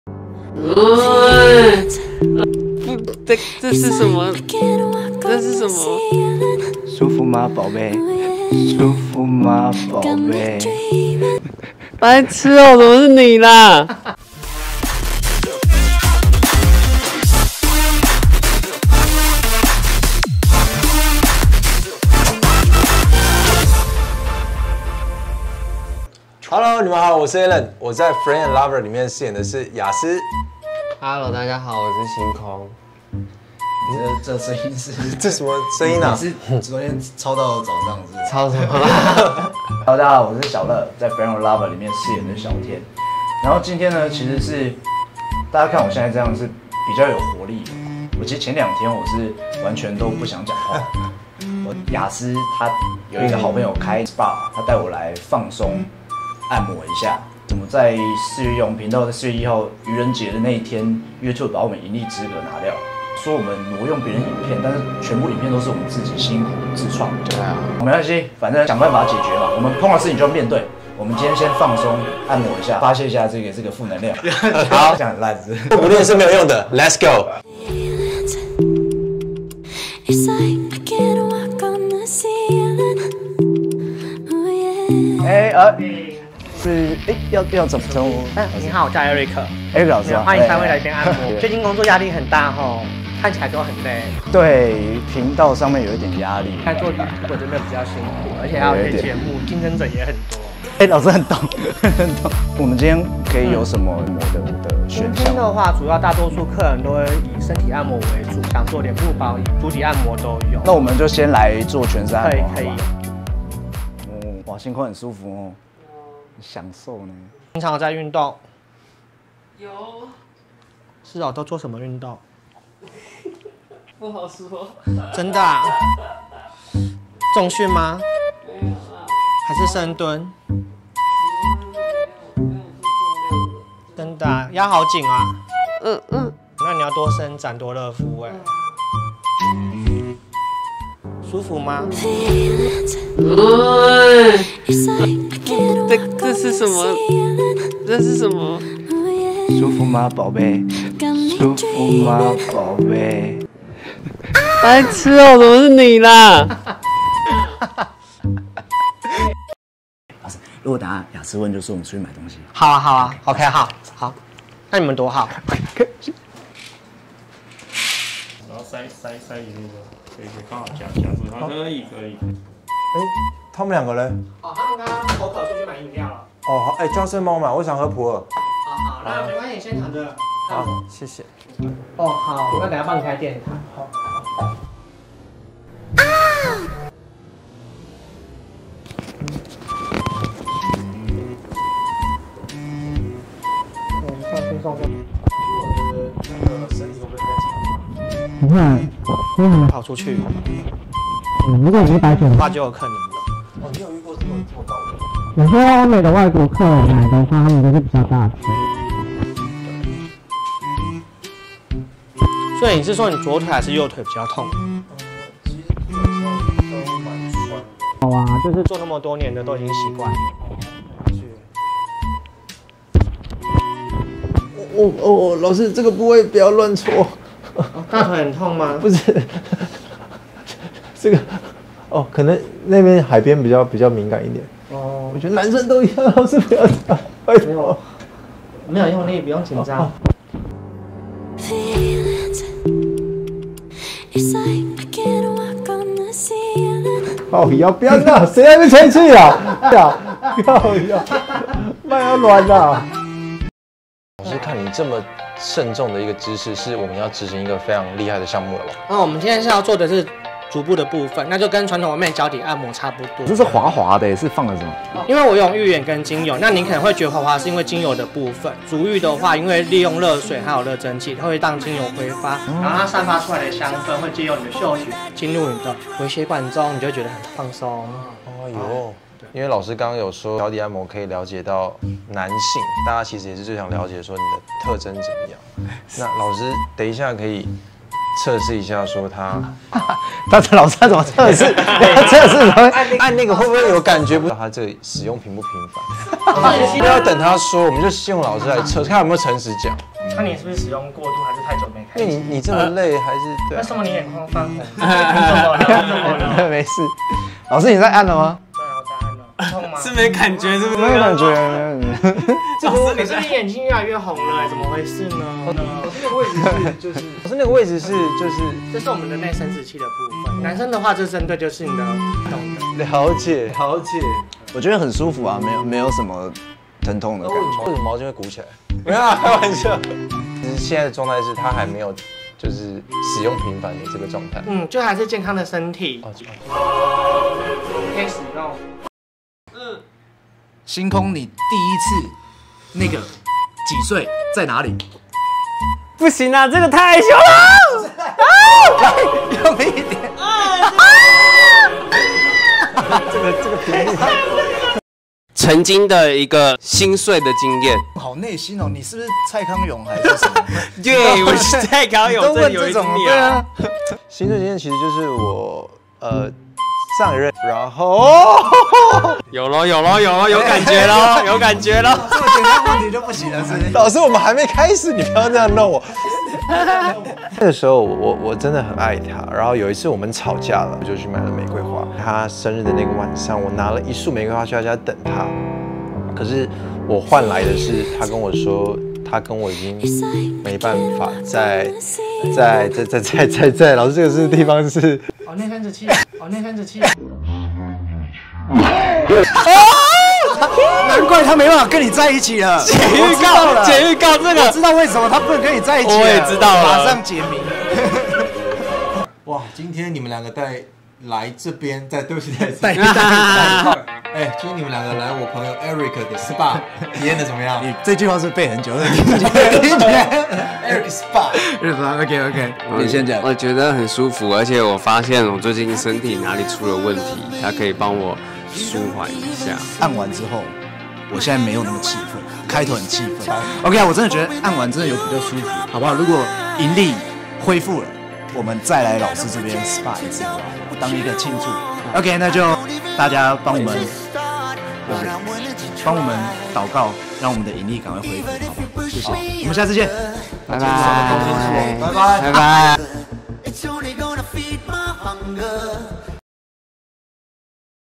最讓人臉紅心跳的按摩體驗!!!! 我跟星空 亞斯的第一次....｜BL vlog｜進擊少年團 小樂 Xiaole Boyslove
因為最近心情不好 亞斯帶我去按摩放鬆一下 沒想到居然是這麼臉紅心跳的體驗. !!??

Because I’m in a bad mood recently, Yas took me to a massage to relax. I didn’t expect it to be such a blushing heartbeat experience...!!??